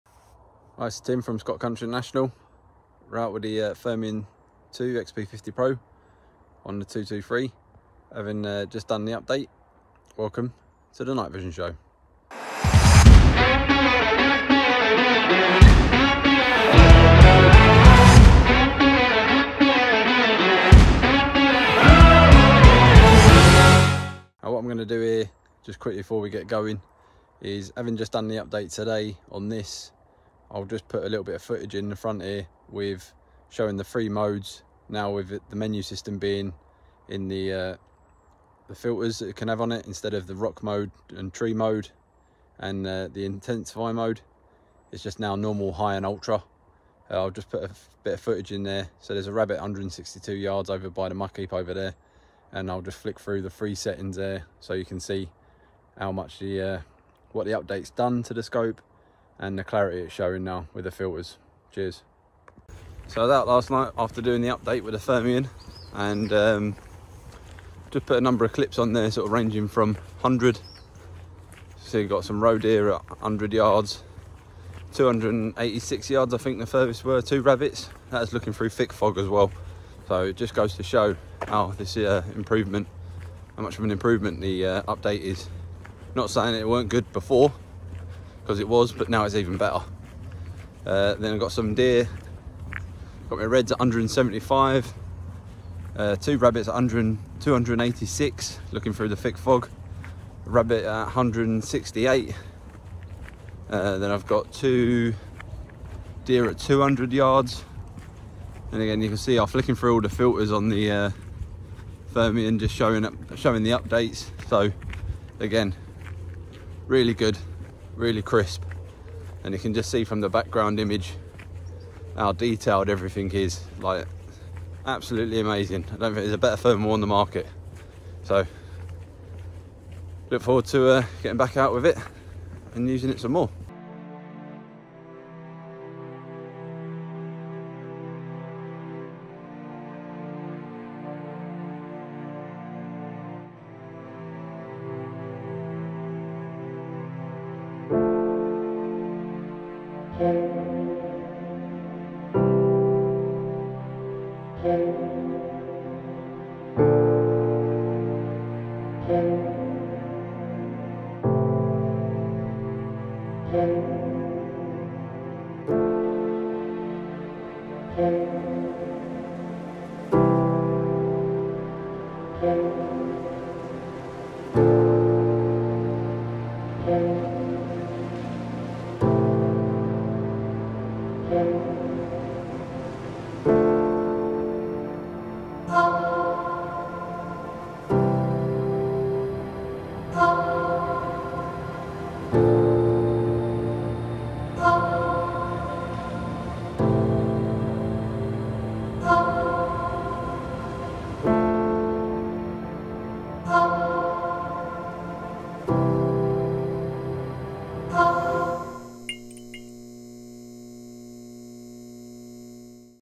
How Good is the new Pulsar Firmware 2.0 for Thermion Pro, Merger etc
Tim has just updated his Thermion 2 PRO to the latest Pulsar Firmware 2.0 which promises "an even more detailed, sharper and informative thermal imaging of complex scenes"

Lets have a look if it lives up to the hype!